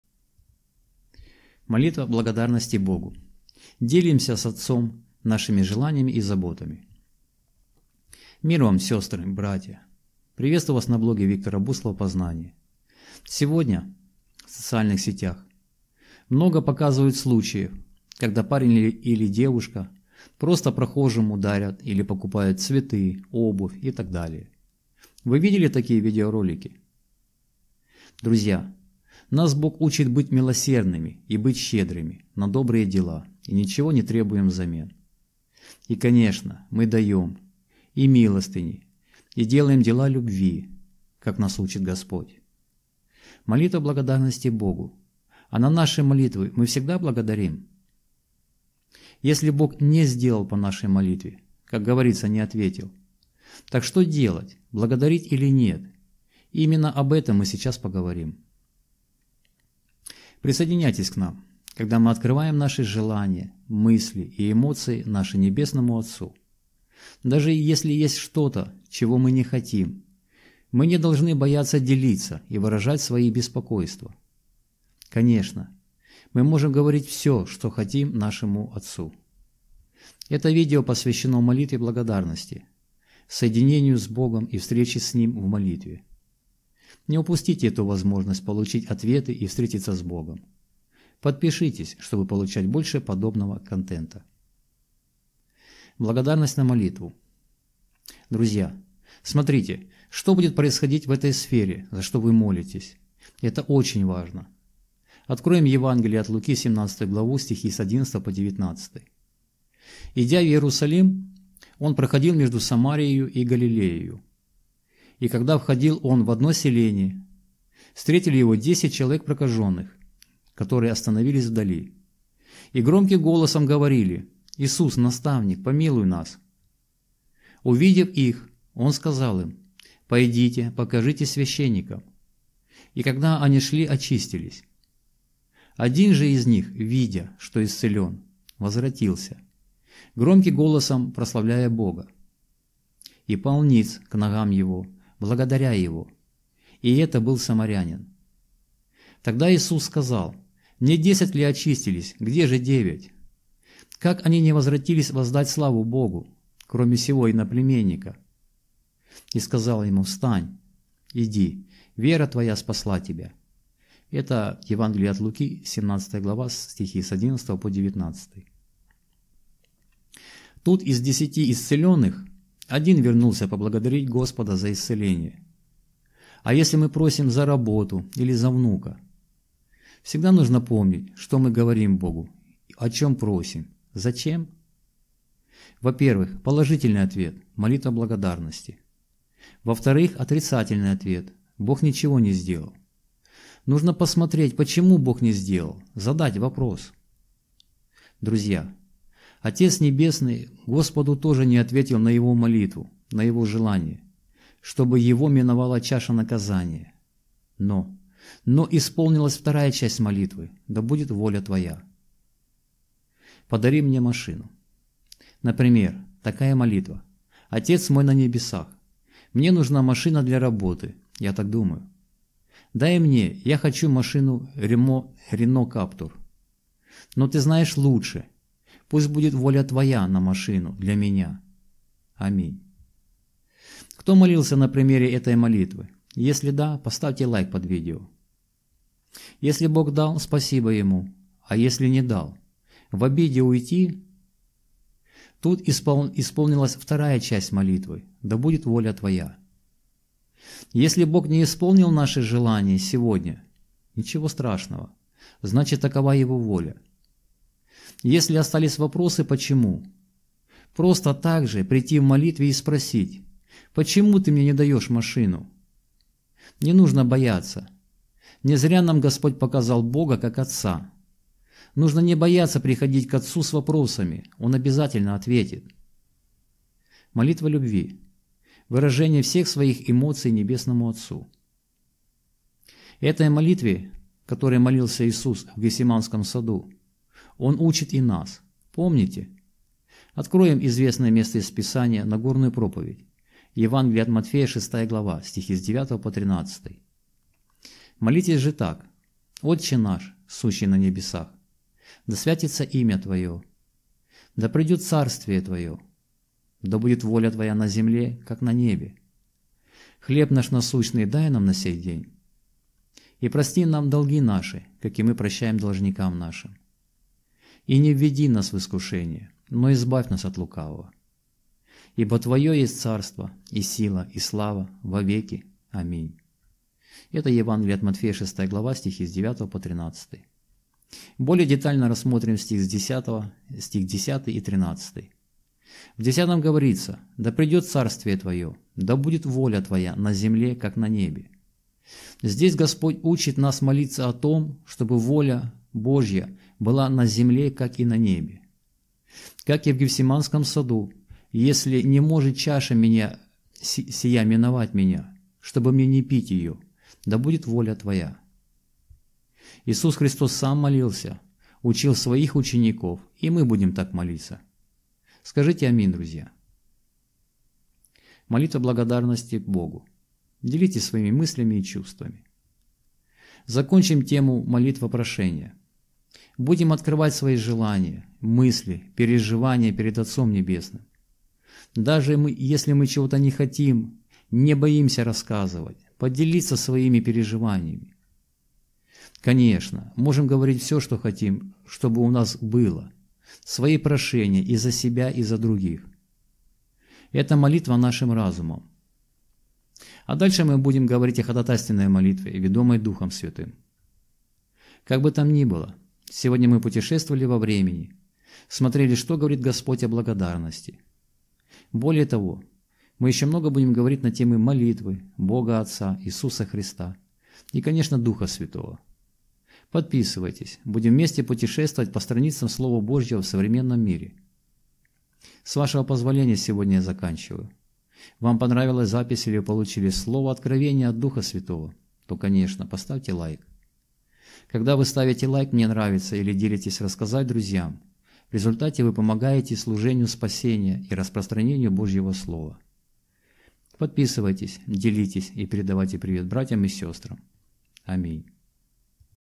Молитва благодарности Богу: делимся с Отцом нашими желаниями и заботами
Присоединяйтесь к нам, в молитве благодарности Богу, когда мы открываем наши желания, мысли и эмоции нашему Небесному Отцу. Даже если есть что-то, чего мы не хотим, мы не должны бояться делиться и выражать свои беспокойства. Конечно, мы можем говорить все, что хотим. Это видео посвящено молитве благодарности, соединению с Богом и встрече с Ним в молитве. Не упустите эту возможность получить ответы и встретиться с Богом.